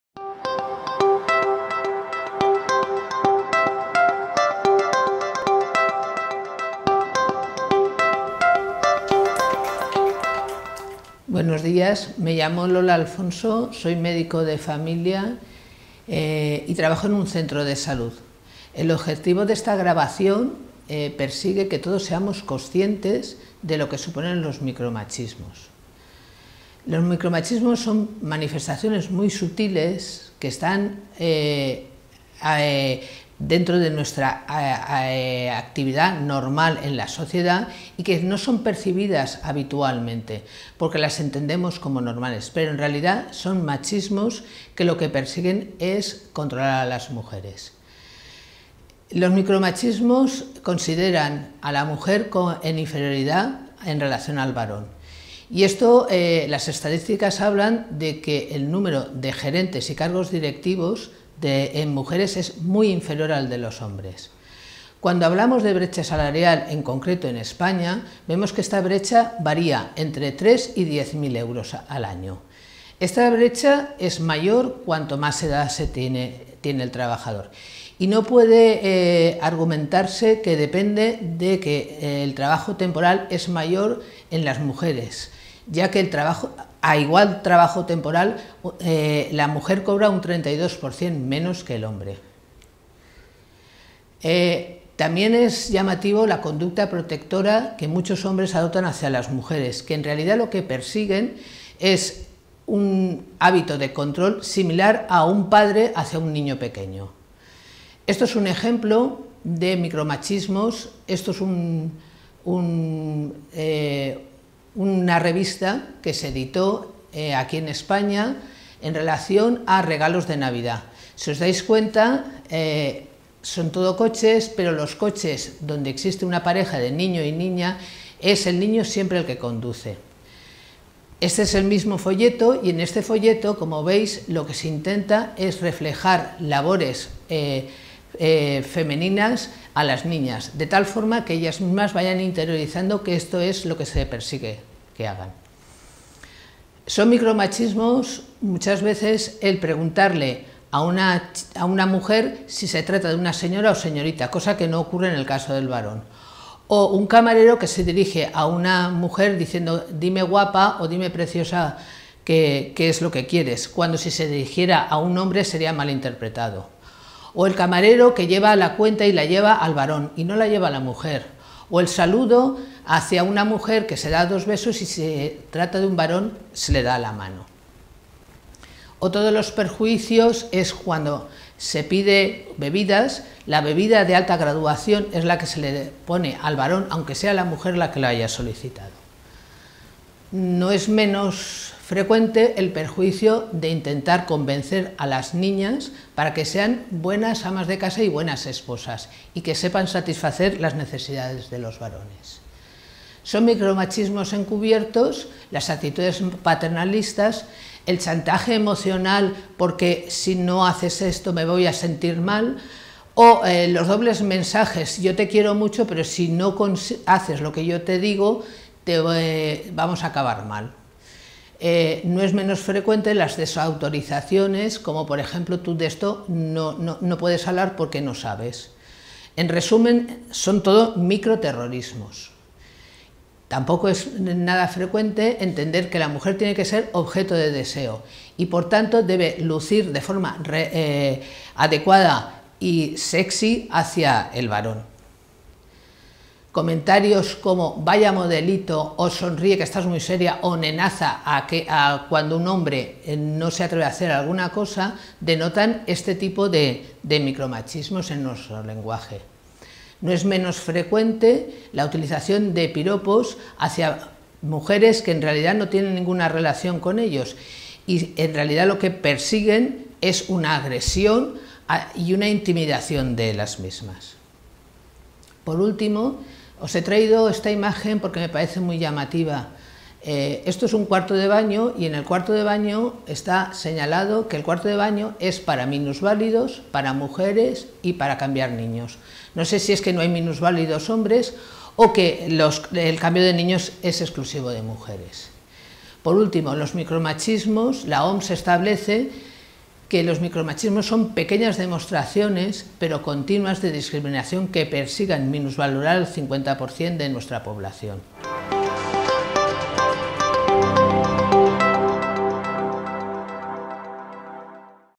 3.3. Micromachismos
Vídeo correspondiente a la 3ª lección del Módulo 3: "Atención a la mujer" del MOOC de "Medicina familiar y atención primaria"